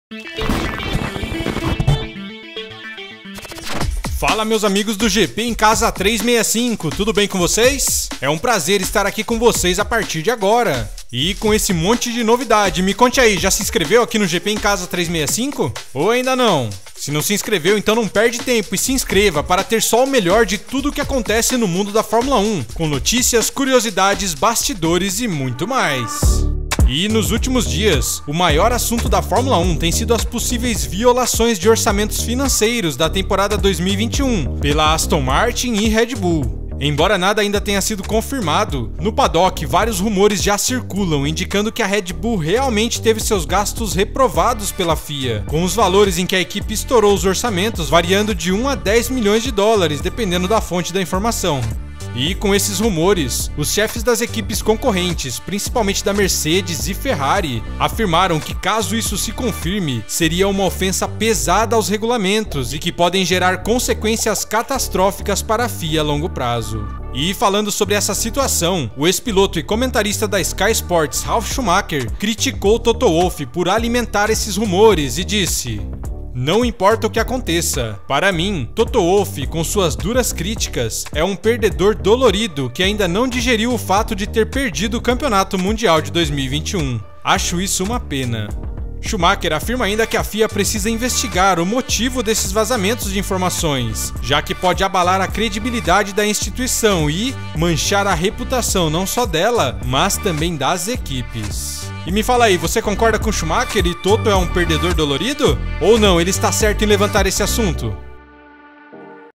SCHUMACHER FAZ DURAS CRÍTICAS À TOTO POR FALAS CONTRA A RED BULL: "PERDEDOR DOLORIDO!" | GPEMCASA365
E com esses rumores, os chefes das equipes concorrentes, principalmente da Mercedes e Ferrari, afirmaram que caso isso se confirme, seria uma ofensa pesada aos regulamentos, e que podem gerar consequências catastróficas para a FIA a longo prazo.

As principais notícias de Fórmula 1, sempre de forma rápida e direta ao assunto: Esse é nosso objetivo aqui no canal GP EM CASA 365.